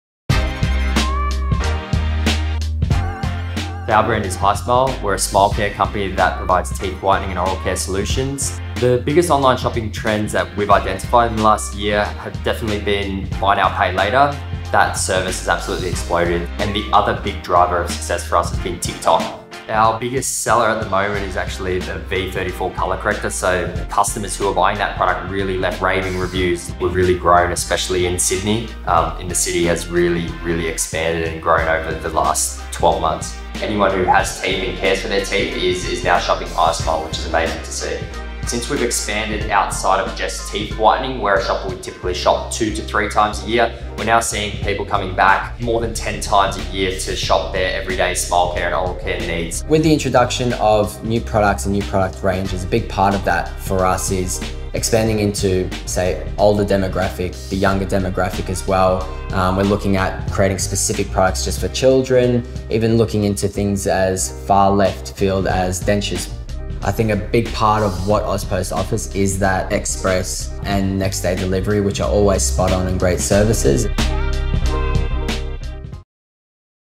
Hismile share their top eCommerce insights
Alex Tomic and Nik Mirkovic, Co-Founders at Hismile share some of their top online shopping insights and trends.

Get all the latest eCommerce trends and insights in our 2022 Inside Australian Online Shopping Report.